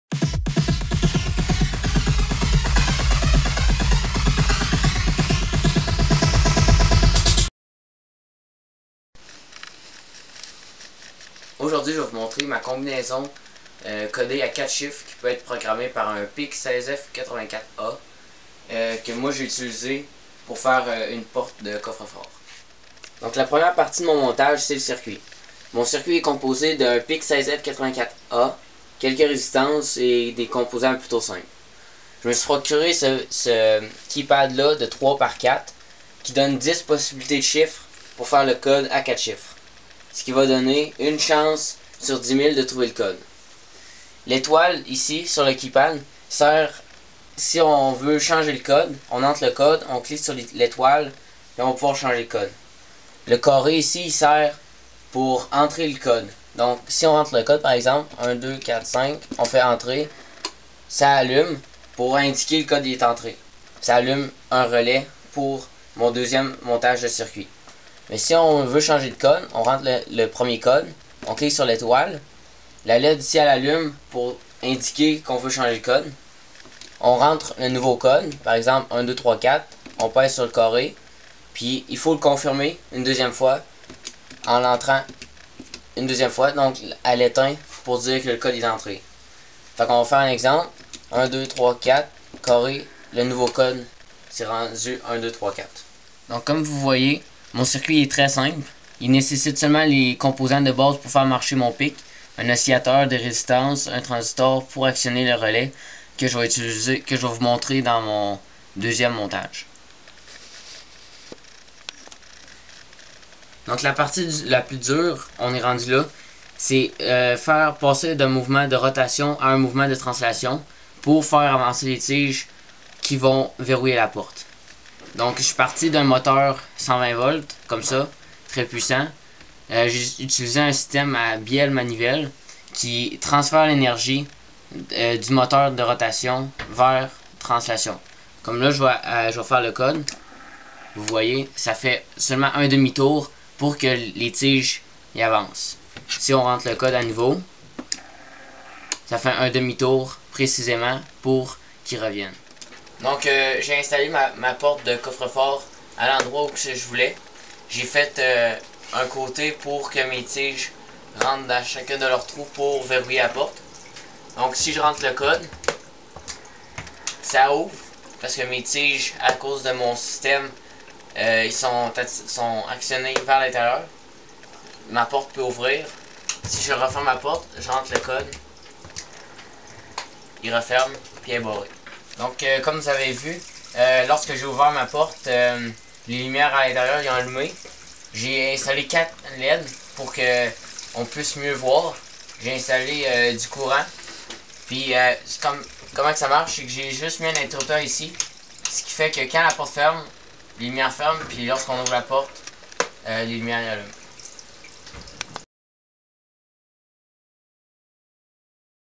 Combinaison codée avec PIC16F84A
Dans cette video, je vous presente ma porte de coffre-fort avec combinaison de 4 chiffres. Le tout est programmé avec un PIC16F84A et j'utilise un keypad 3x4 pour l'interface numerique. J'explique aussi comment verrouillé la porte avec un mouvement.